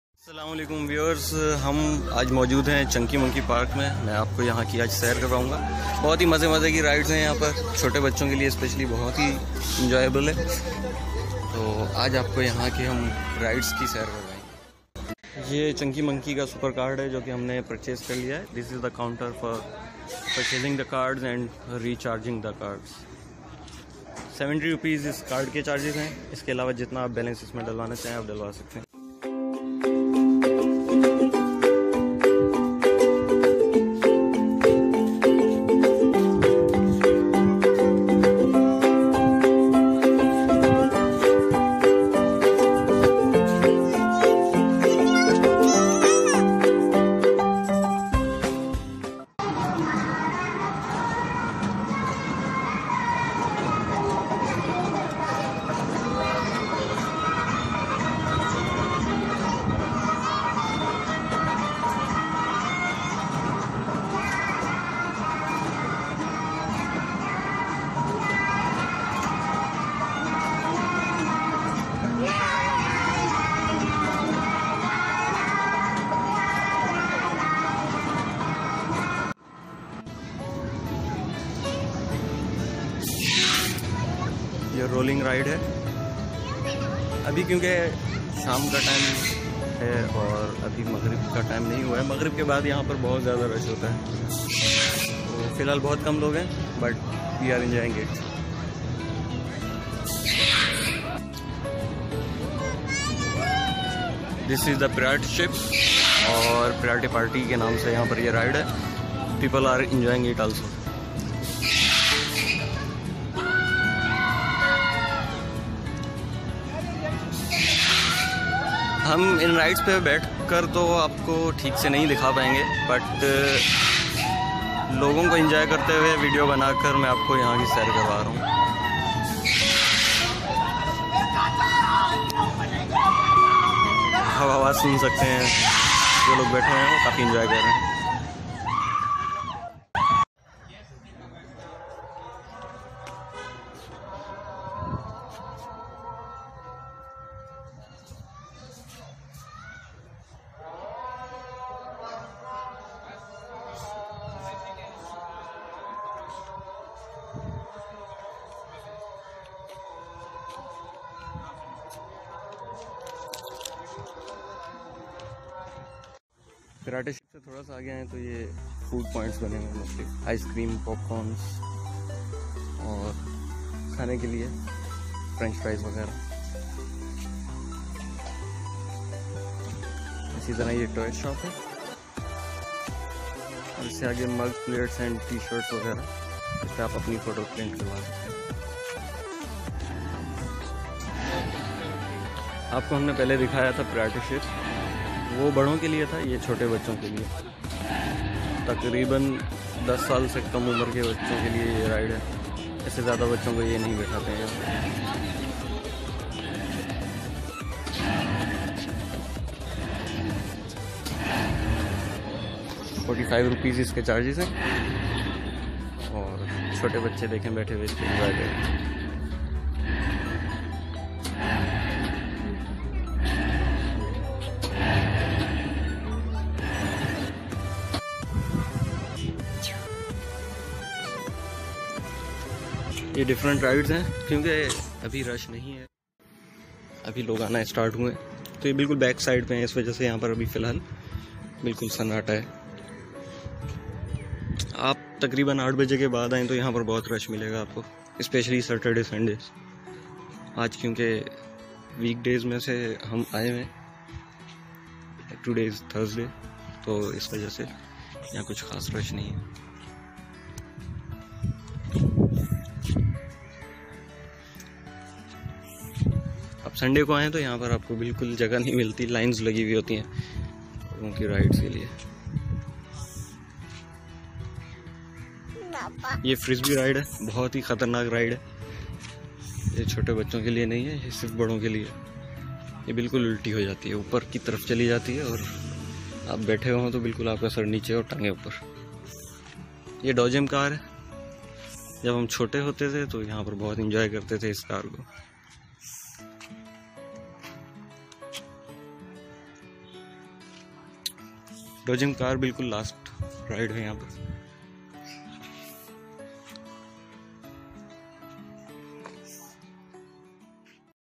Complete trip to Chunky monkey Karachi Pakistan... Vlog
We have started to make videos of different places of Pakistan, its our first video at chunky monkey amusement park Karachi Pakistan.